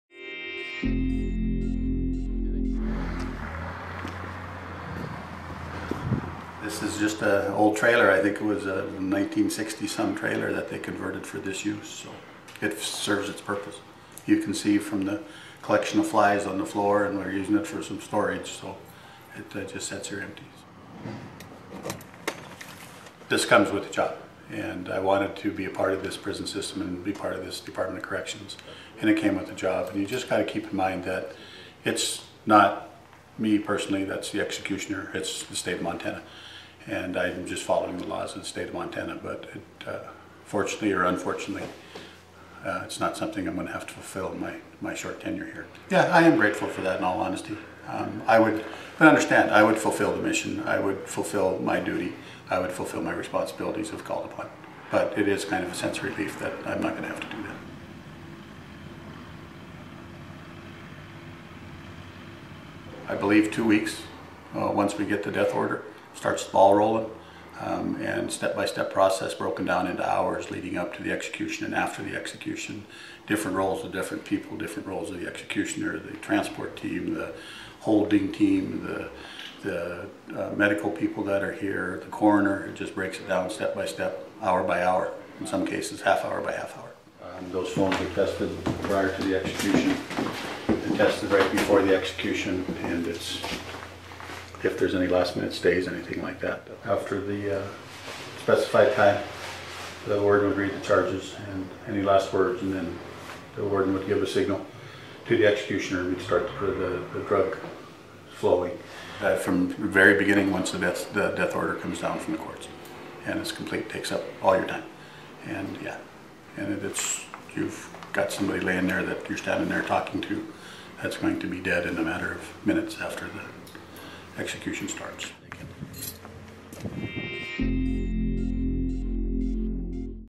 Inside the execution chamber at Montana State Prison
Montana State Prison warden Leroy Kirkegard says it's a "relief" he may never oversee an execution, but adds he would do the job if called upon. There is a stay on all executions in Montana, stemming from a court challenge. (Oct. 16, 2016)